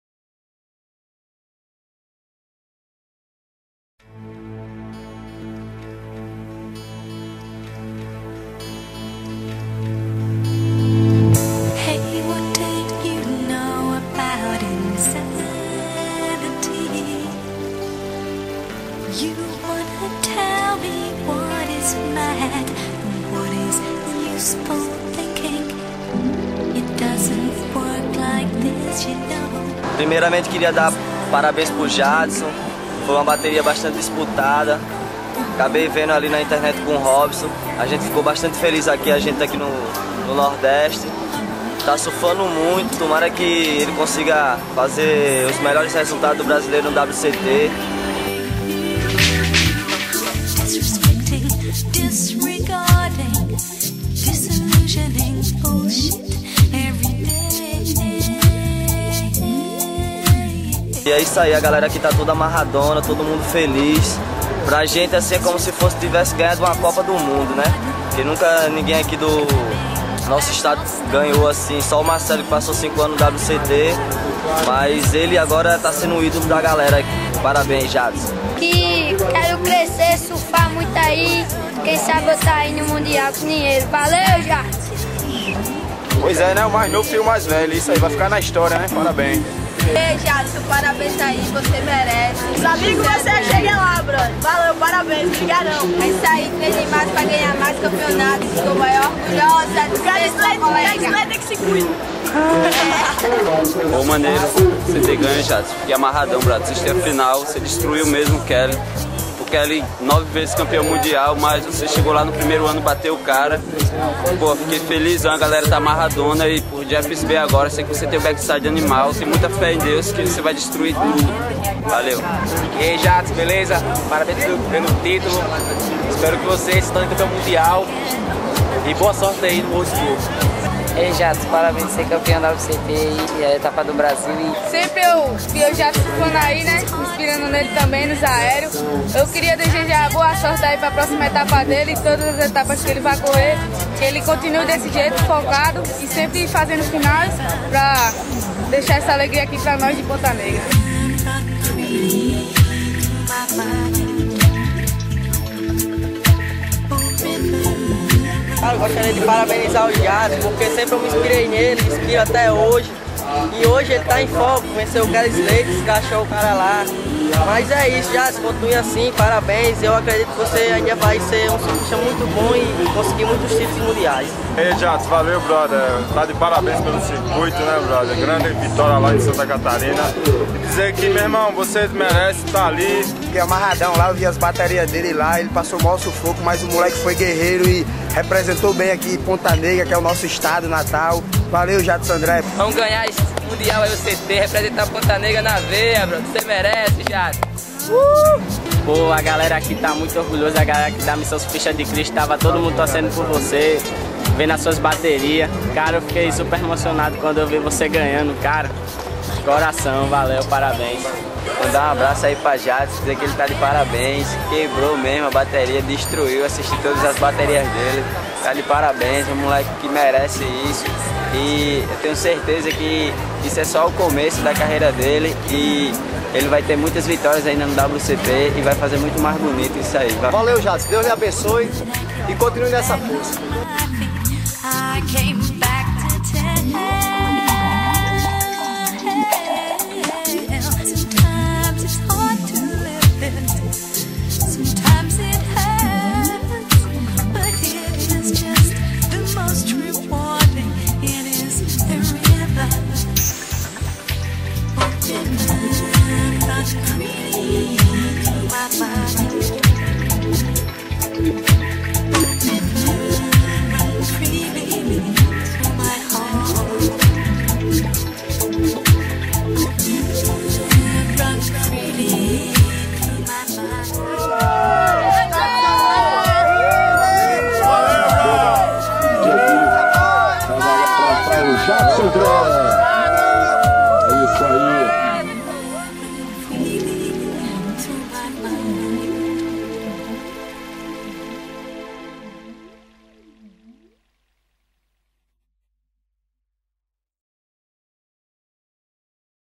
Vidasurf - Ponta Negra homenageia Jadson André.wmv
A galera de Ponta Negra esta muito orgulhosa de Jadson andré e o parabeniza por mais essa conquista, vancendo Kelly Slater na etapa brasileira do mundial de surf.